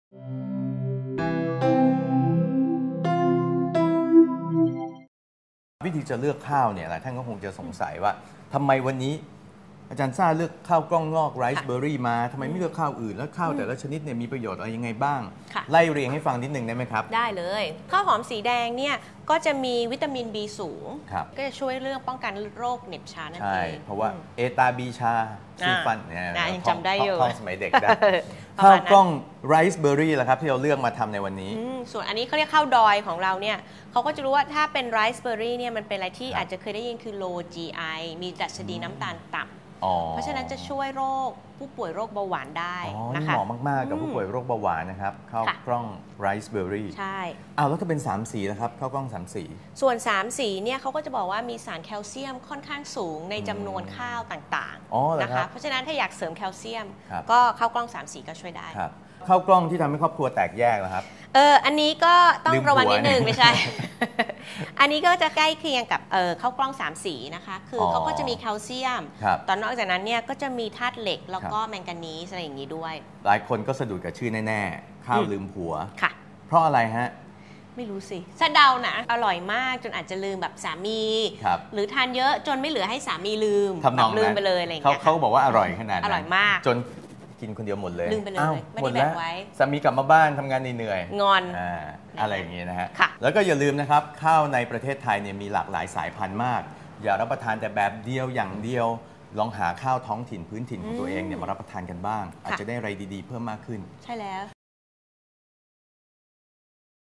[Clip] Healthy Friday [by Mahidol] ข้าวกล้องต่างชนิด คุณค่าทางโภชนาการ ต่างกันหรือเปล่า
เดี๋ยวนี้ ข้าวกล้องในท้องตลาดมีให้เลือกซื้อหลากหลายสายพันธุ์ ทั้งข้าวกล้องสีแดง ข้าวกล้องไร์เบอร์รี ข้าวกล้อง 3 สี ข้าวลืมผัว แค่ 4 ชนิดนี้ แม้จะเป็นข้าวกล้องเหมือนกัน แต่คุณค่าทางสารอาหารจะแตกต่างกันหรือไม่ ฟังคำอธิบายจาก อ.ซ่า-ดร.อลิสา นานา นักกำหนดอาหารและอาจารย์ประจำวิทยาลัยวิทยาศาสตร์และเทคโนโลยีการกีฬา ม.มหิดล

Healthy Friday : ข้าวกล้องพิชิตโรค